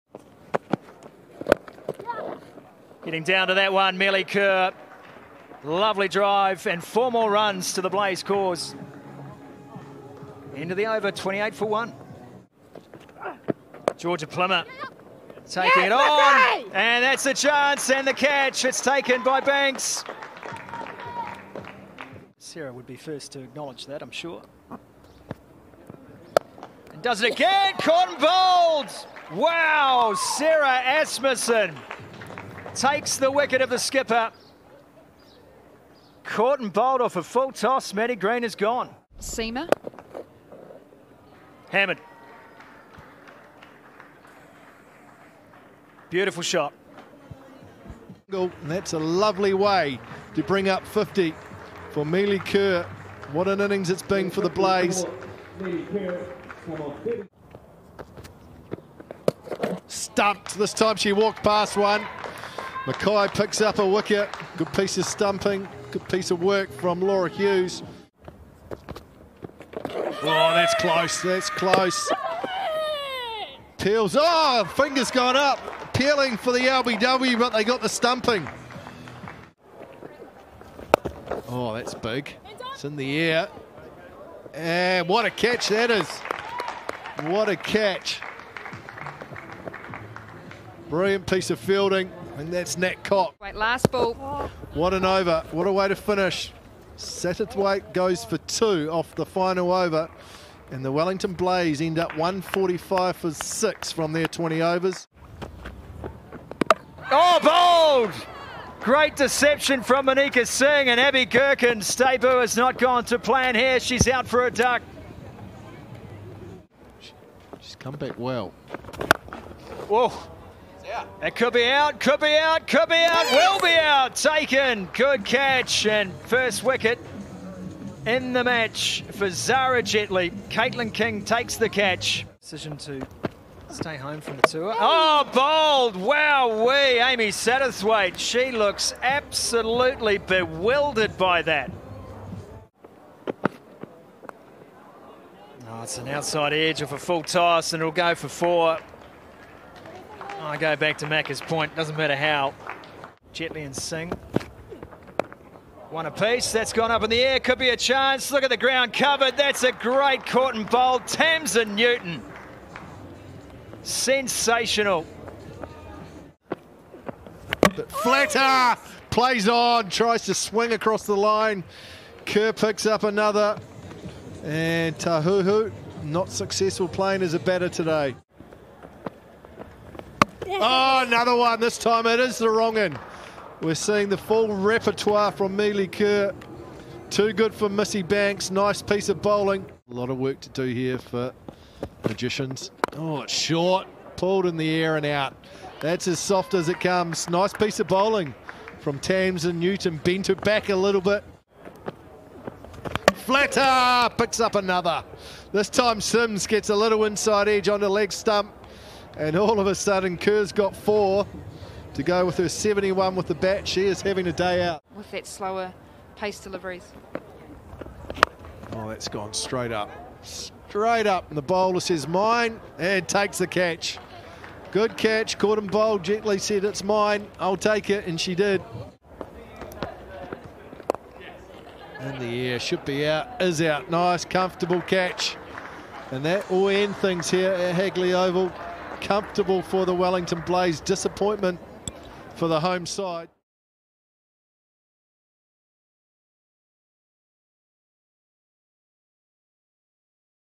Canterbury Magicians v Wellington Blaze MATCH HIGHLIGHTS | Hagley Oval - Dream11 Super Smash 2021-22
Highlights of the Canterbury Magicians and Wellington Blaze Dream11 Super Smash match at Hagley Oval.